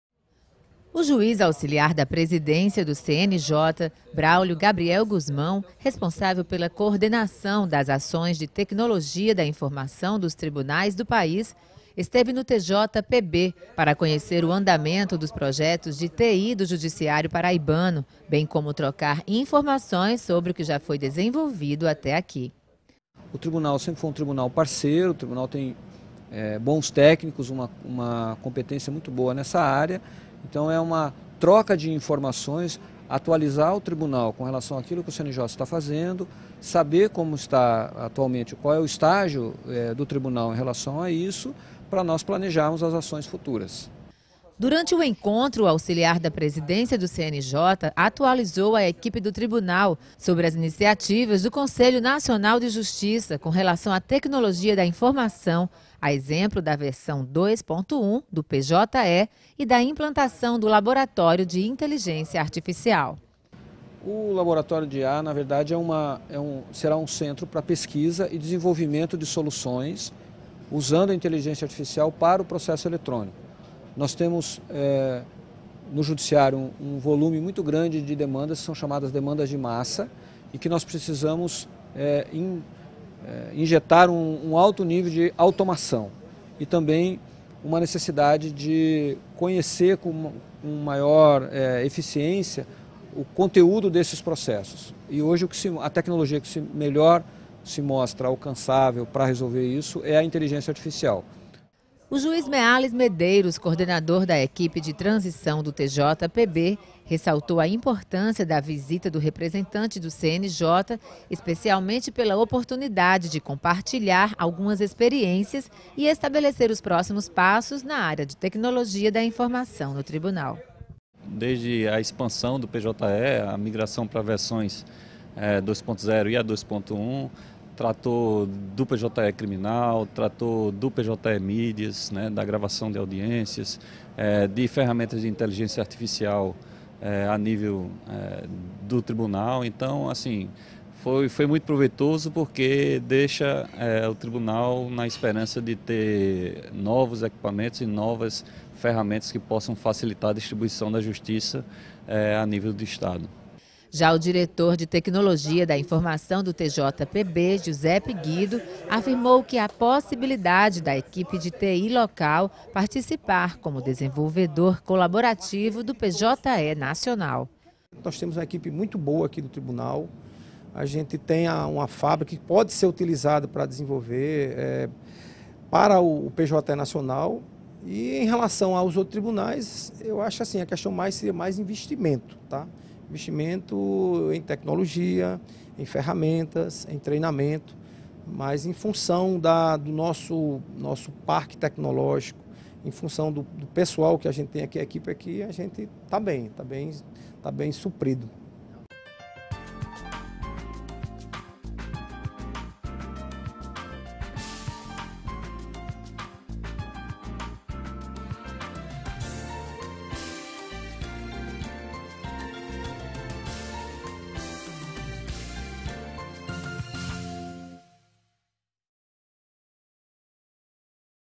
Juiz auxiliar do CNJ Visita ao TJPB 12 12 2018
Juiz auxiliar da Presidência do CNJ visita TJPB para conhecer ações na área de TI